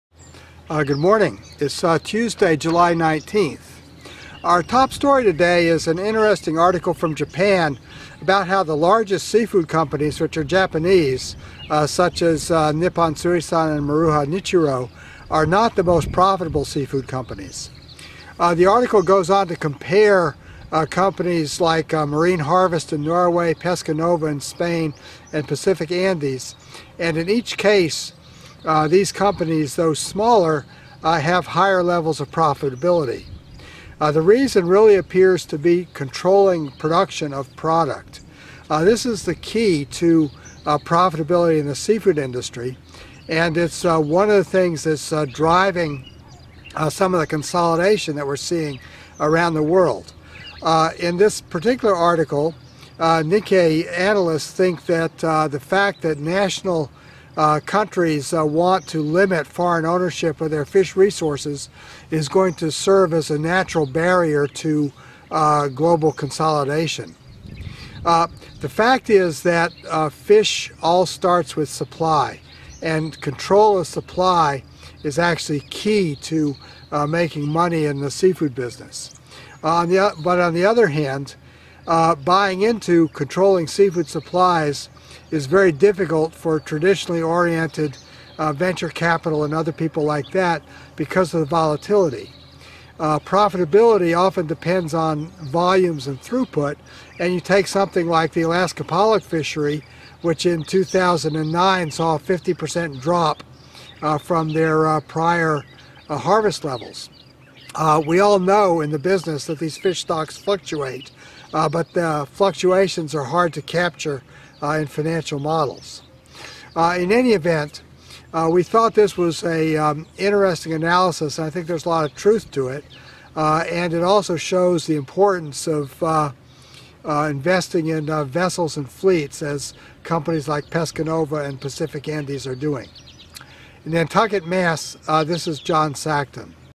Japan has biggest seafood companies, but not the most profitable
Japanese seafood companies are the largest in the world but not the most profitable as they do not control sources of fish supply. By contrast companies like Marine Harvest Pescanova and Pacific Andes have invested in supply and show a higher level of profitability.